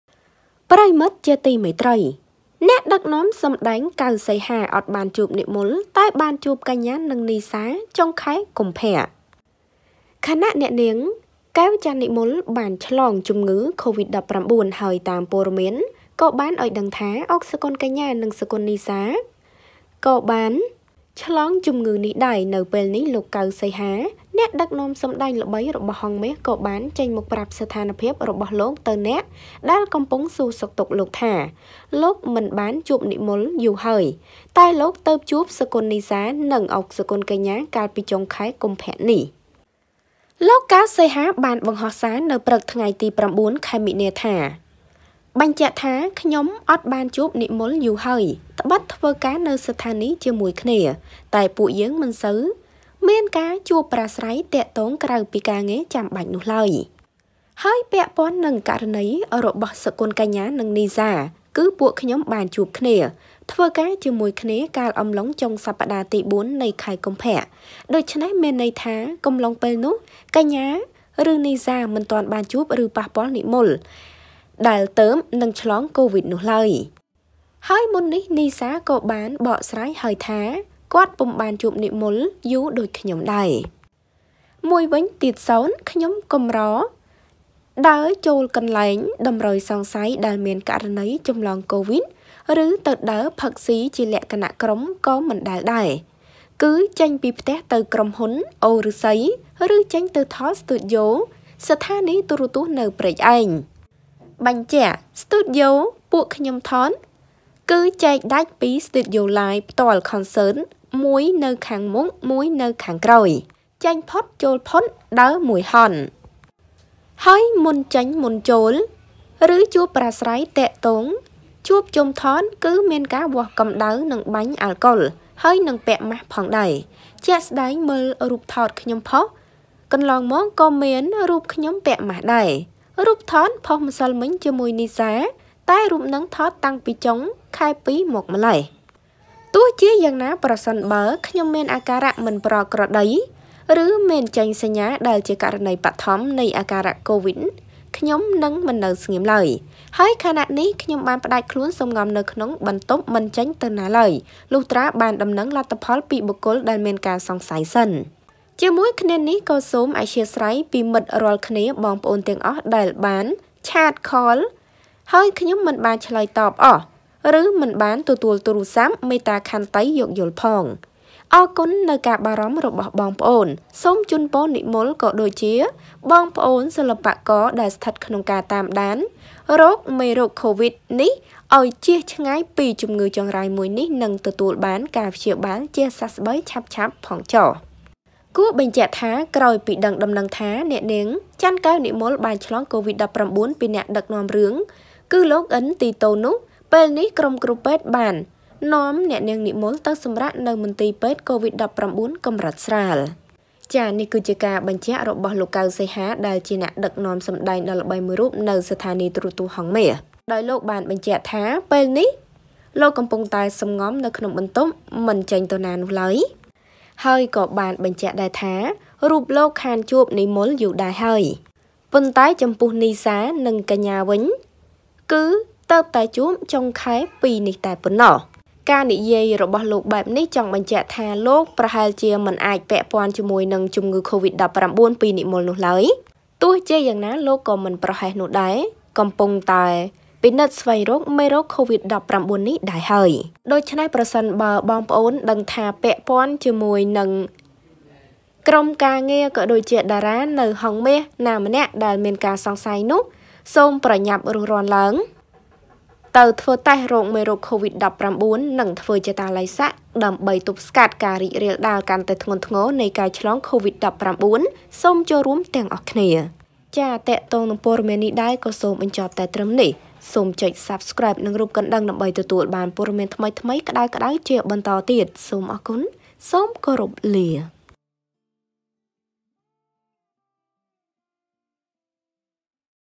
កាន់តែរីកធំ! អ្នកដឹកនាំសម្តែង កៅ សីហា ចេញបញ្ជាក់ ស្ថានភាពខ្លួន ពាក់ព័ន្ធកញ្ញា និងនីសា មិនជួបនិមល
សូមអធ្យាស្រ័យ

ចំពោះការមិនបានតប ខំមិនបានគ្រប់គ្នា

Ms Socheata